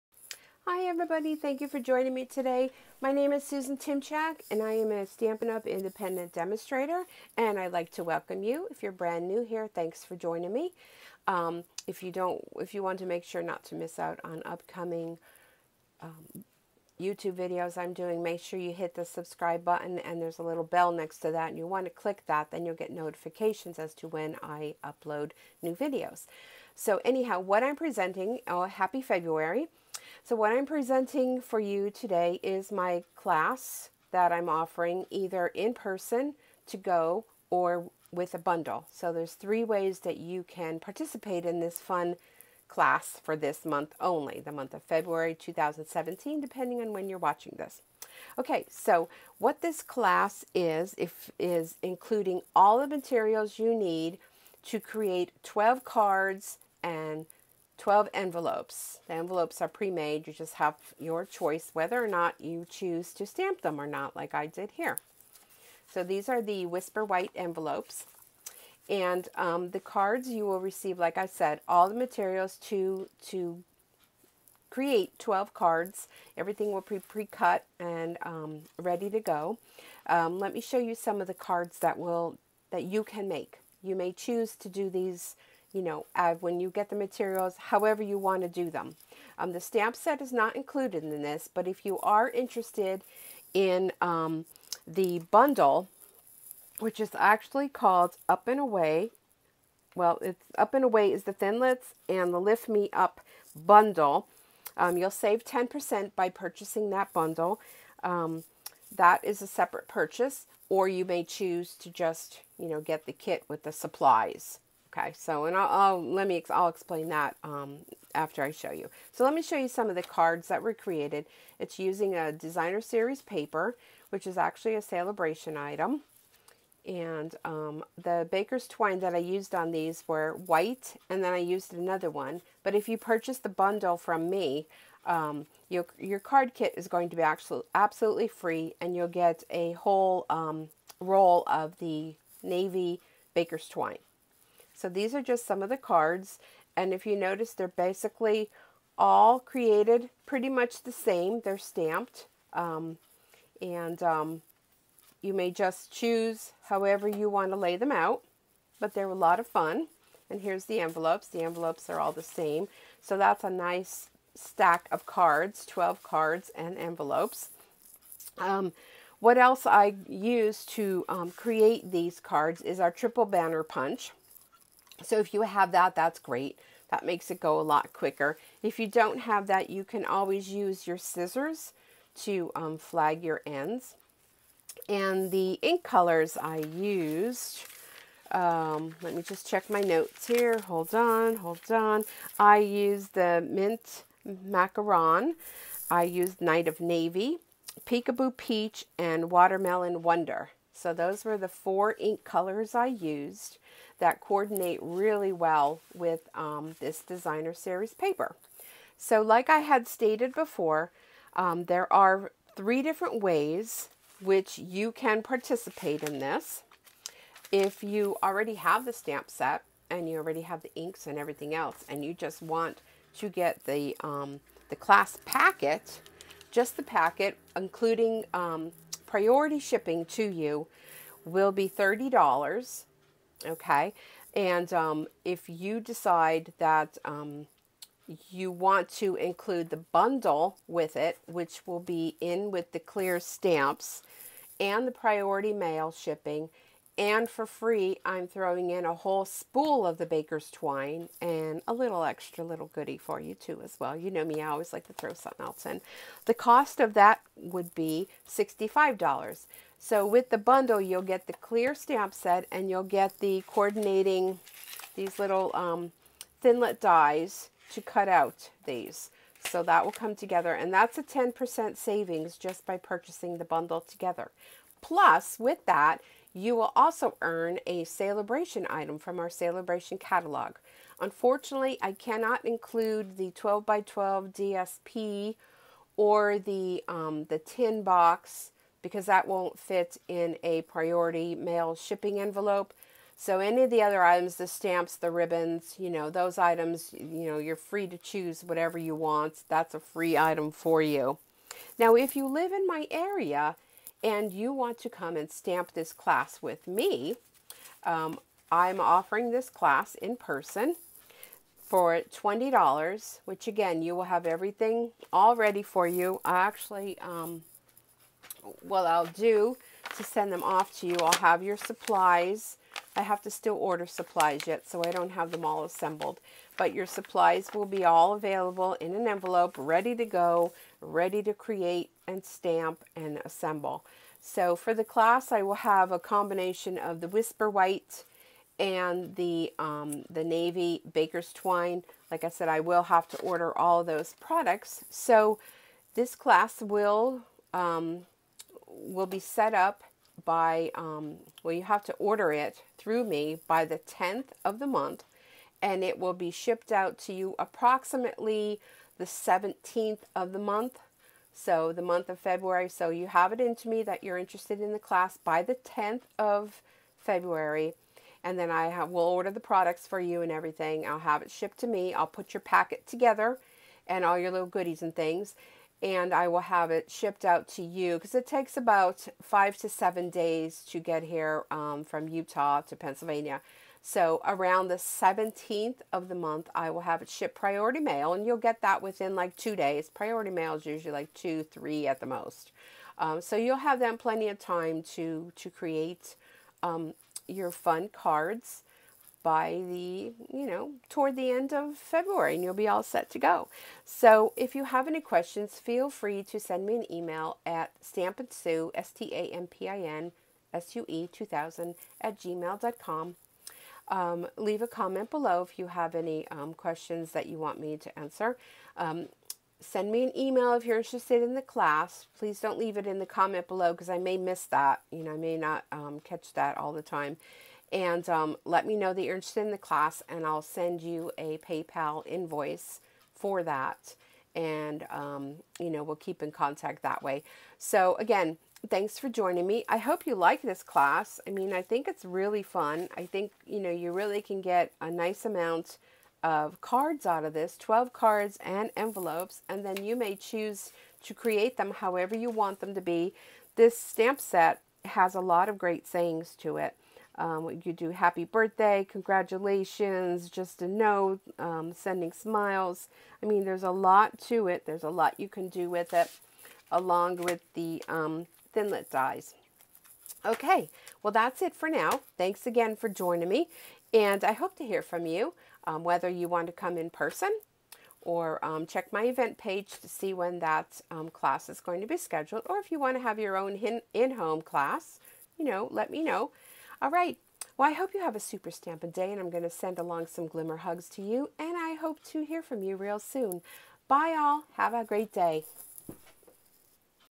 February Class Lift Me Up by Stampin' Sue Creates
February 2017 Class in the Mail
You may chose option 1-just class packet.  2-class w/ bundle and goodies or 3- stamp with me in person.